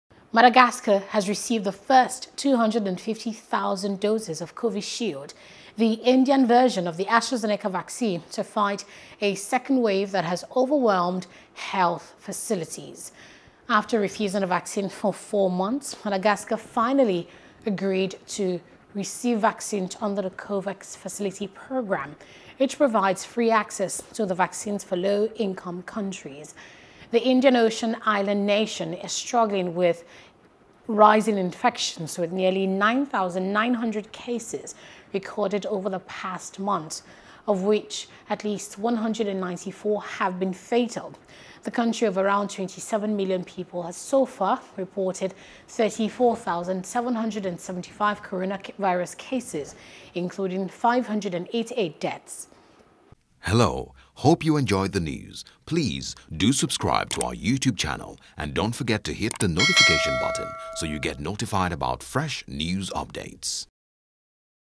COVID-19: Madagascar Receives First Batch Of Vaccines | AFRICAN
Madagascar has received the first 250,000 doses of Covishield - the Indian version of the AstraZeneca vaccine - to fight a second wave that has overwhelmed health facilities.

After refusing the vaccine for four months, Madagascar finally agreed to receive vaccines under the Covax facility program, which provides free access to the vaccine for low-income countries.